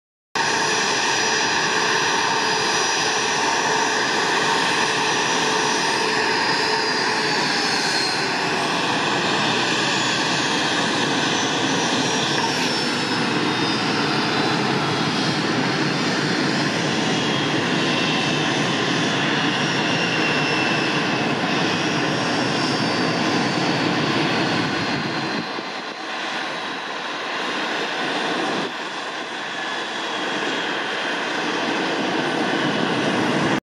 Wellington Airport | Garden City Helicopters | Bombardier Challenger 604 (ZK-JCJ) engine shutdown
Flight Info :

Flight no - ZKJCJ
Callsign - N/A
Route - Wellington ~ Kerikeri
Departure time - 1:16 pm
Arrival time - 2:21 pm
Total flight time - 01:05
Cruise altitude - 35,000 ft